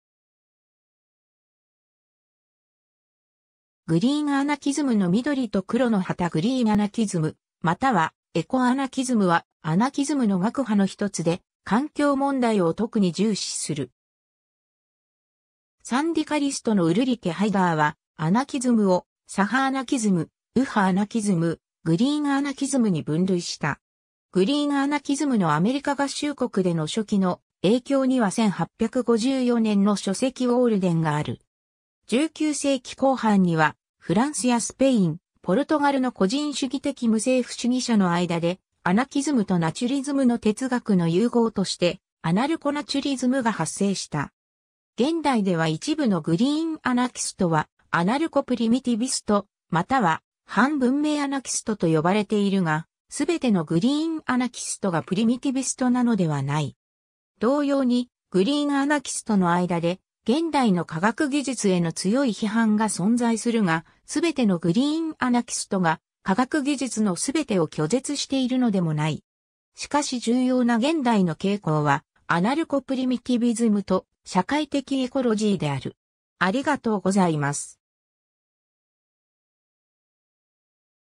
グリーンアナキズム
グリーンアナキズムの緑と黒の旗 グリーンアナキズム（英語: Green anarchism）またはエコアナキズム（英語: ecoanarchism）は、アナキズムの学派の1つで、環境問題を特に重視する。
 サンディカリストのUlrike Heiderはアナキズムを、左派アナキズム、右派アナキズム（無政府資本主義）、グリーンアナキズムに分類した。
 グリーンアナキズムのアメリカ合衆国での初期の影響には1854年の書籍 Walden (en)がある。
19世紀後半にはフランスやスペイン、ポルトガルの個人主義的無政府主義者の間で、アナキズムとナチュリズムの哲学の融合としてアナルコナチュリズム(en)が発生した。
 現代では一部のグリーンアナキストはアナルコプリミティビスト（無政府原始主義者）または反文明アナキストと呼ばれているが、全てのグリーンアナキストがプリミティビストなのではない。
同様にグリーンアナキストの間で現代の科学技術への強い批判が存在するが、全てのグリーンアナキストが科学技術の全てを拒絶しているのでもない。
しかし重要な現代の傾向はアナルコプリミティビズムと社会的エコロジーである。